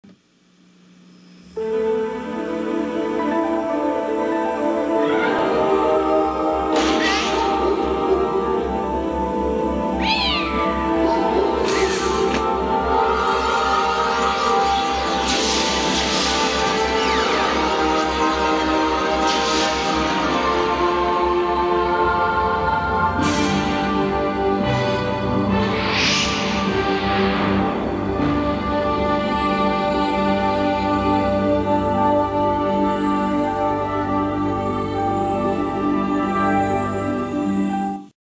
THE DISNEY CHANNEL, Mickey Mouse UFO, flying cat, bumper used in 1994 & 1995
Jan. 8, 1995. Pulled from my recent Disney promos block and boosted with an audio upgrade using an alternate stereo source track blended in with my own mono one. Disney meets the X-Files/Sightings Alien mania of the mid-1990s. Love it. Enjoy being a kid again.